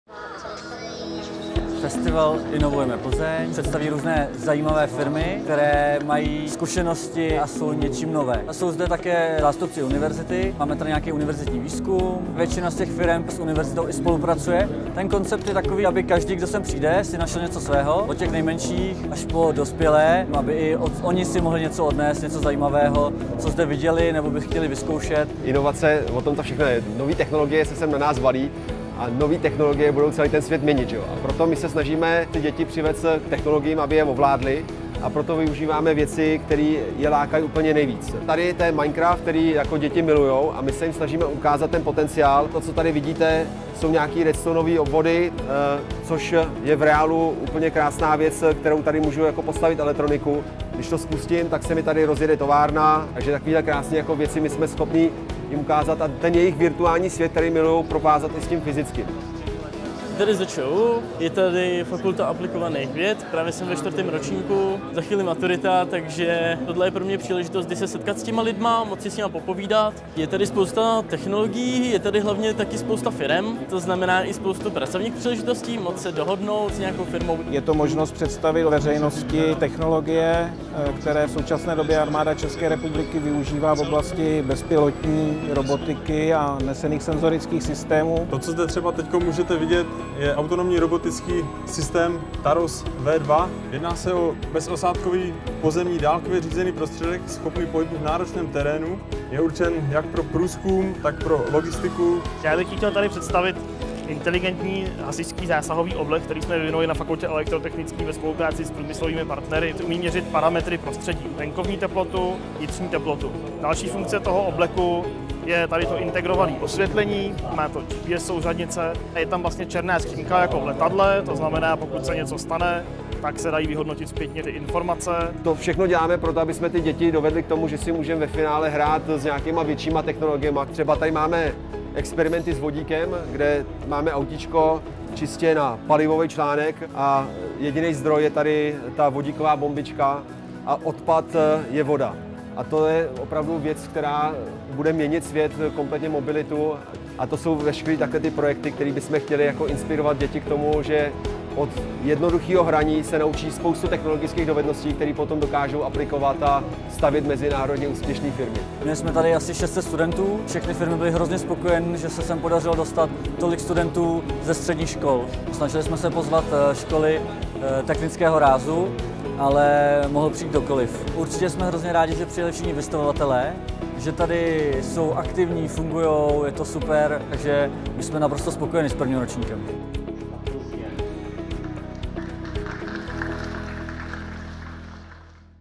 FESTIVAL INOVUJEME PLZEŇ 2018
Video reportáž z festivalu Inovujeme Plzeň.
Vyrobeno pro: Správa informačních technologií města Plzně
Vyrobila: Video Factory Production